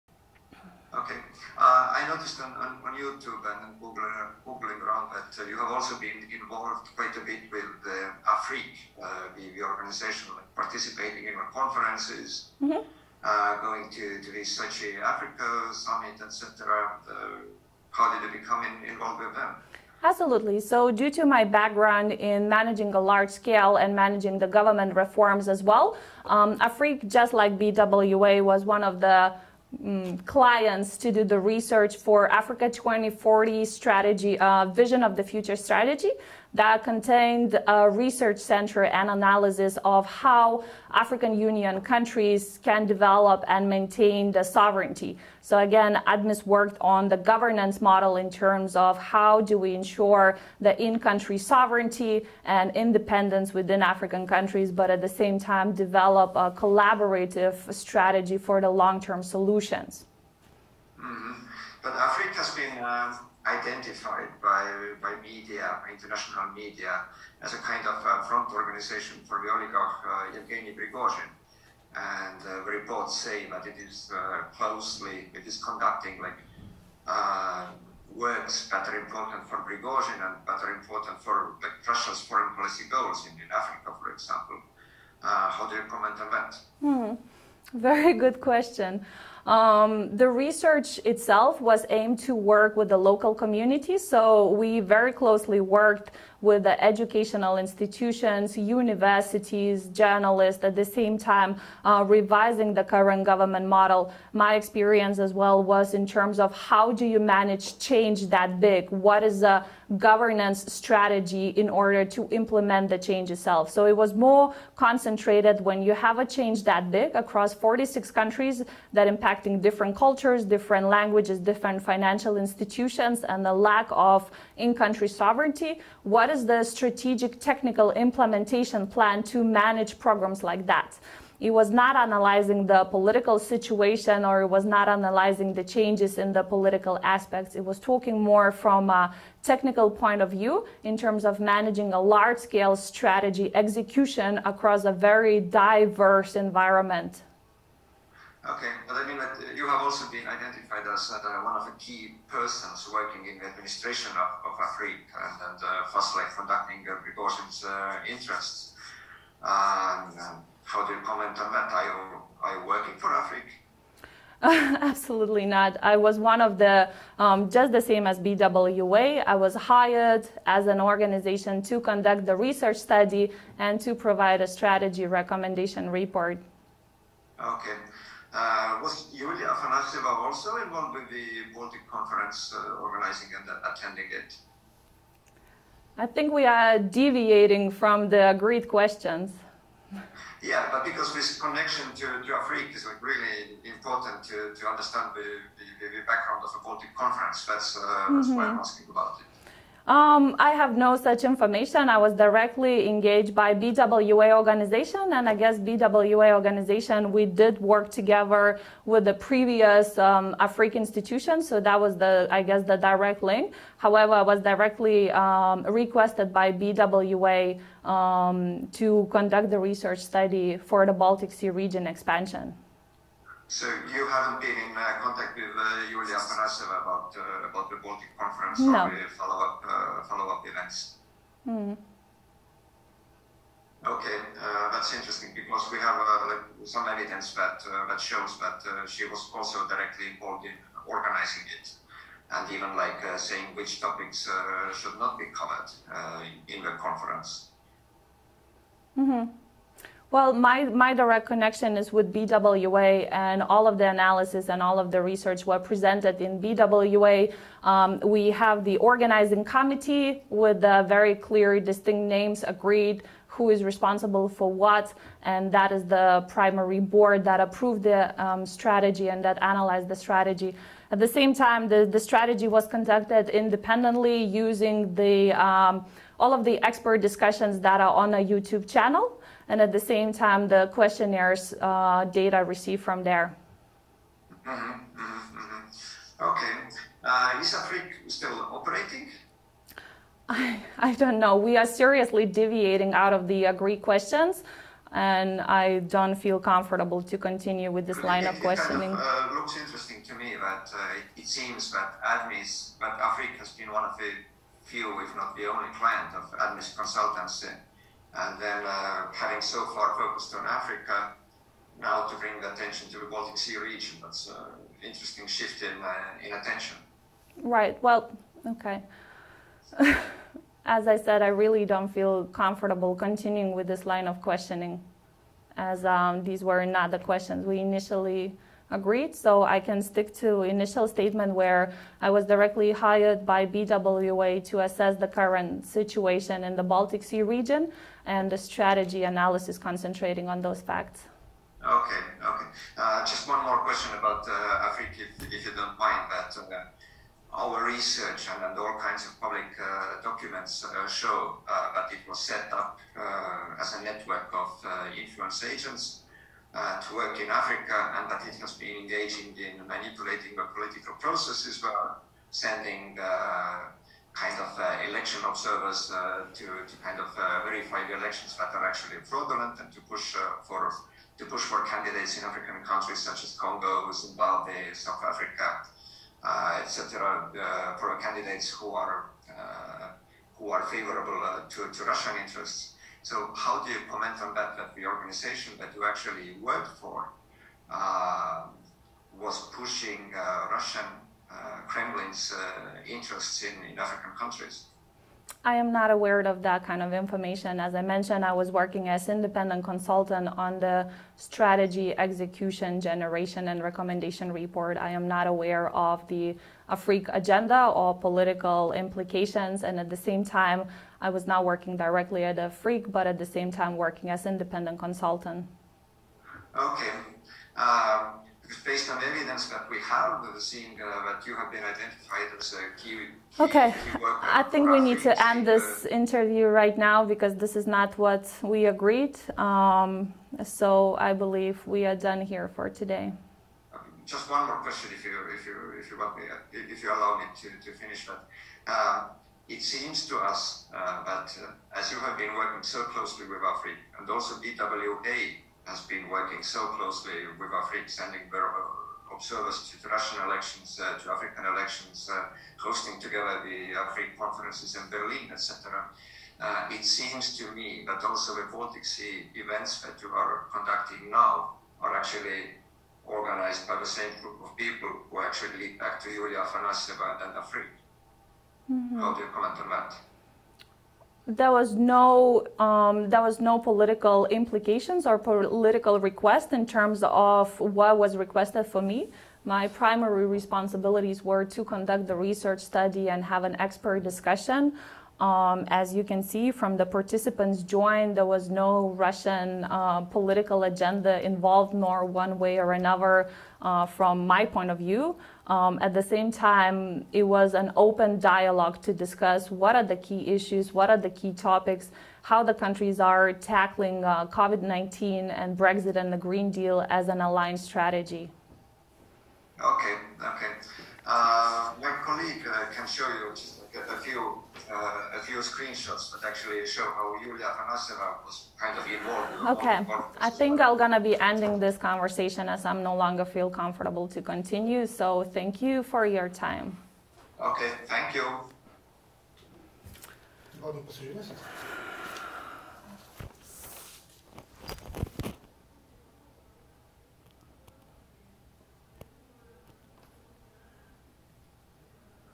Lietuviški „Putino virėjo“ pėdsakai: Delfi.ee interviu su Vaiva Adomaityte
Jevgenijus Prigožinas, taip pat žinomas kaip „Putino virėjas“ FTB ieškomas dėl kišimosi į JAV rinkimų sistemą. ES jam pritaikė sankcijas dėl ryšių su „Wagner“ samdiniais, sėjančiais chaosą karo zonose. Jis siejamas su vadinamaisiais trolių fabrikais ir laikomas vienu artimiausių Vladimiro Putino patikėtinių. Ir iki jo veda, iš pirmo žvilgsnio, jokių prokremliškų tendencijų nerodantys renginiai, organizuojami iš Vilniaus.

Pokyčių eksperte prisistatanti Vaiva Adomaitytė kurį laiką organizuoja diskusijų sesijas, orientuotas į aktualias europinės politikos temas – nuo aplinkosaugos iki pandemijos. Į renginius kviečiami skirtingų Europos šalių politikai ir verslininkai.

Tarp kviestinių svečių iš Lietuvos – buvusi aplinkos viceministrė Justina Grigaravičienė ir neseniai paskirtas užsienio reikalų viceministras Arnoldas Pranckevičius. Pastarasis savo dalyvavimą atšaukė likus porai dienų iki renginio – bet ne todėl, kad įtarė kažką negero.

Renginiai orientuoti į europinę darbotvarkę, kviečiami gerą reputaciją ir pripažinimą turintys kalbėtojai, organizatoriai – verslo asociacija iš Vokietijos ir V.Adomaitytės įmonė. Tačiau jungtinis „The Daily Beast“, centro „Dossier“, Vokietijos transliuotojo ARD, Delfi.ee ir tiriamosios žurnalistikos centro „Siena“ tyrimas rodo, kad už kadro stovi J.Prigožino aplinka.

Kaip rodo nutekėję dokumentai, su J.Prigožino operacijomis Afrikoje tiesiogiai dirbusi Julija Afanasjeva tyliai stovėjo už renginių, organizuojamų iš Vilniaus, ir diktavo jų darbotvarkes. „Dossier“ gauti dokumentai rodo, kad Vilniuje dirbanti renginių organizatorė J.Afanasjevą vadino savo „krikštamote“.

Interviu su Delfi.ee žurnalistu V.Adomaitytė neigė bet kokį J.Afanasjevos prisidėjimą prie savo renginių. Paprašyta pakomentuoti „Dossier“ dokumentus, diskusijų organizatorė nutraukė interviu.

Operatorius: Robertas Narmontas / Delfi TV